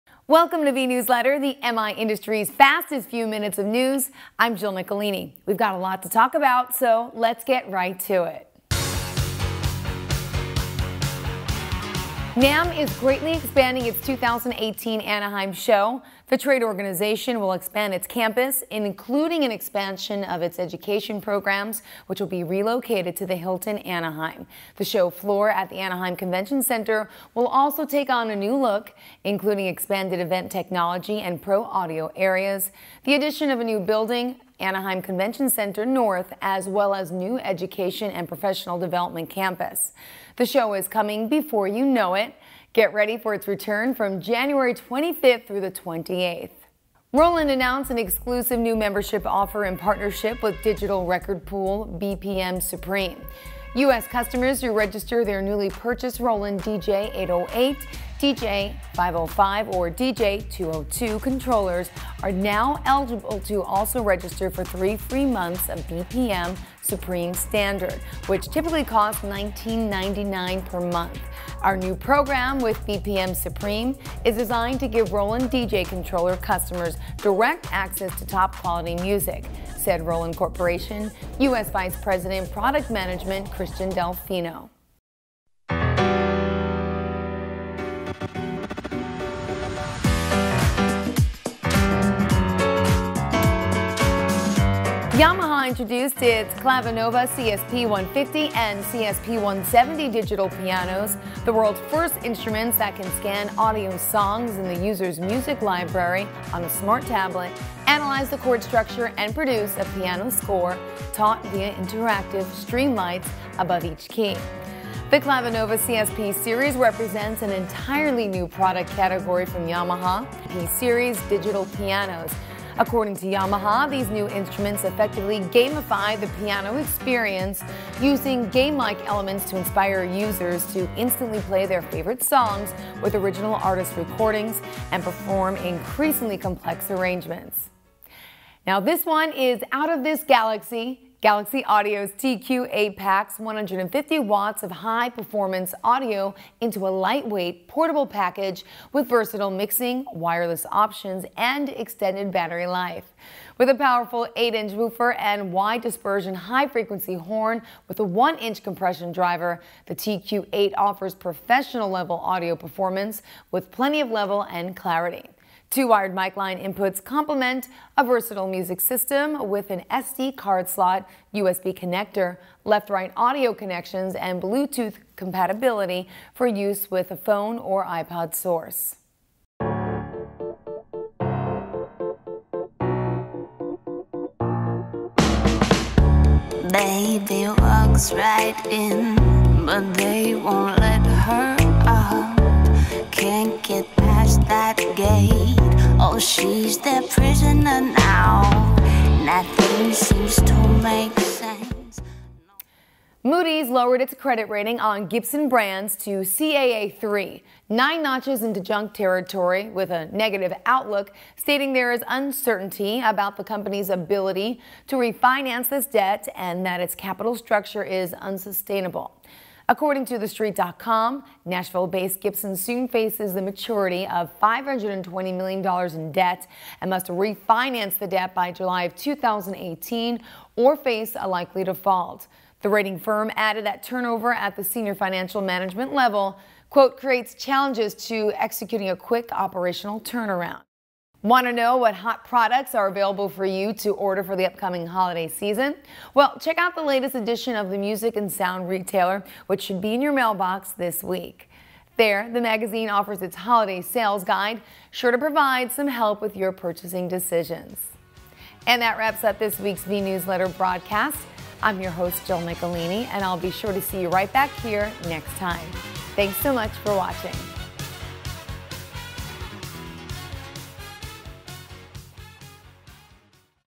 Vnewsletter - September 14, 2017 with Jill Nicolini
Episode #307: Gibson Financial Update

NAMM Show Makes Big Changes
Yamaha Enters New Market
Roland Adds 'Supreme' Offering
Out of This Galaxy
The Holiday Sales Guide is Fast Approaching